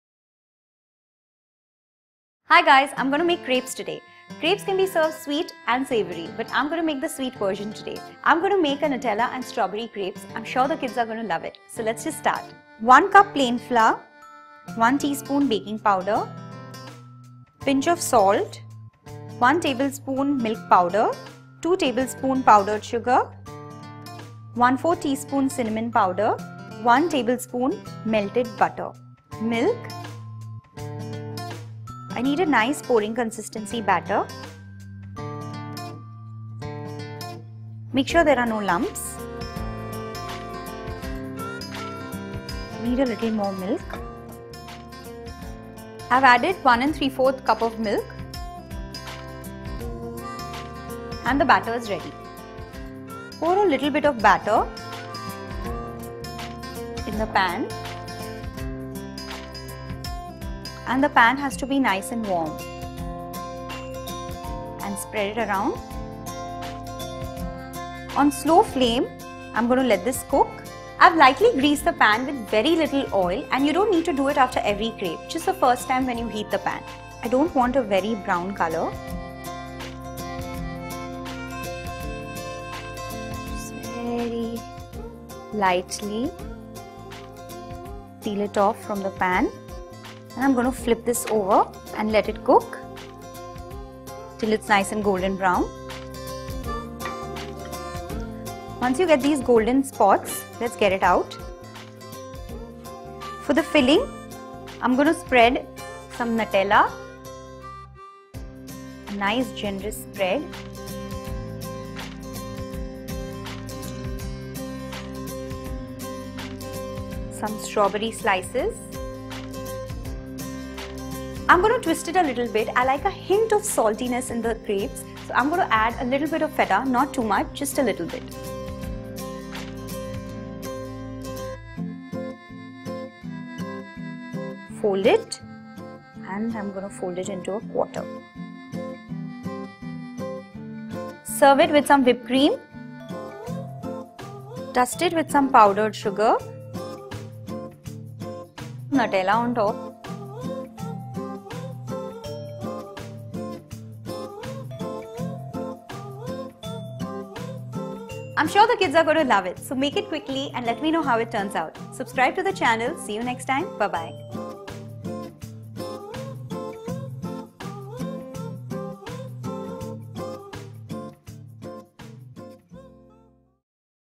Eggless Nutella & Strawberry Crepes | French Sweet Recipe By Ruchi Bharani
Learn how to make Eggless Nutella & Strawberry Crepes - Easy and Quick French Sweet Recipe

One of the easiest and quickest recipe ever with all the sweetness of chocolate and goodness of strawberry, here is a sweet French snacks recipe called Nutella & Strawberry Crepes. Its a very simple and easy to make quick homemade sweet snacks or dessert recipe.

Ingredients:
1 cup Plain Flour
1 tsp Baking Powder
Salt
1 tbsp Milk Powder
2 tbsp Powdered Sugar
1/4 tsp Cinnamon Powder
1 tbsp Melted Butter
1 & 3/4 cup Milk
Nutella
Strawberry Slices
Feta
Whipped Cream

Method:
- Add flour, baking powder, pinch of salt, milk powder, powdered sugar, cinnamon powder, melted butter, milk in a bowl and whisk everything well to form a nice pouring consistency.
- Brush some oil on a frying pan and make a flat pancake out of the mixture
- Apply nutella on the thin pancake and add some sliced strawberry and feta and fold it to make a quarter
- Serve it with some whipped cream, dusted with some powdered sugar and drizzle nutella on top.